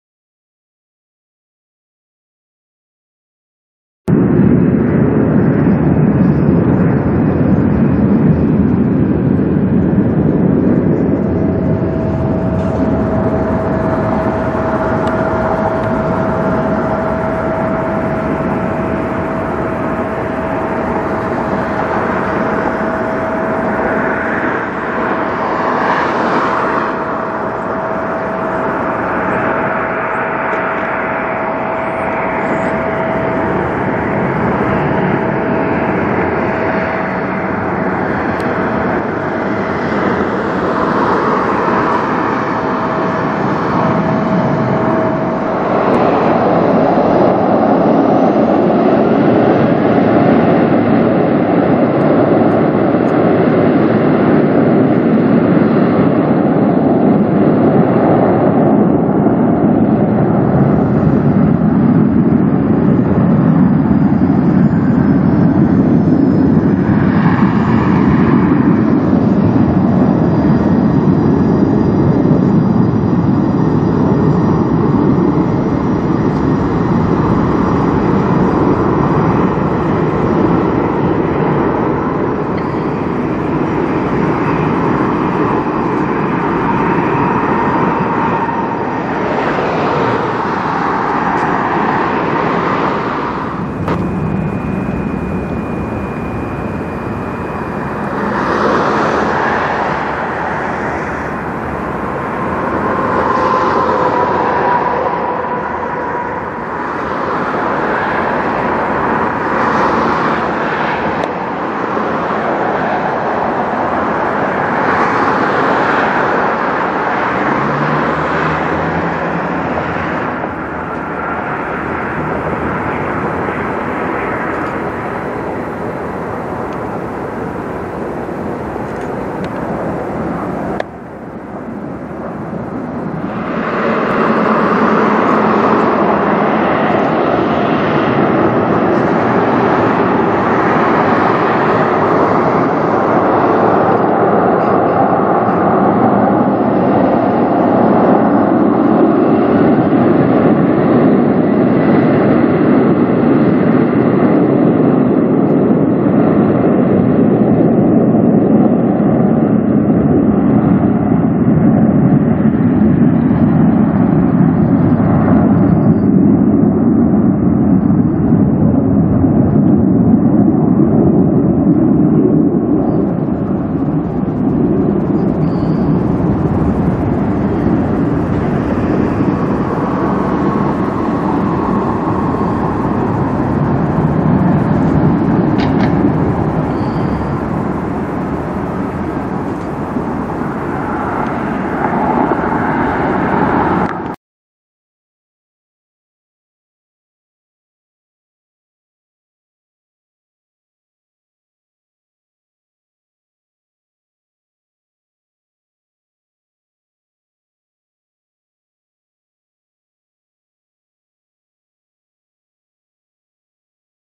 2 American Eagle Landing & 2 Delta Jets Taking Off At Detroit Metro Airport
2 American Eagle Landing & 2 Delta Jets Taking Off At Detroit Metro Airport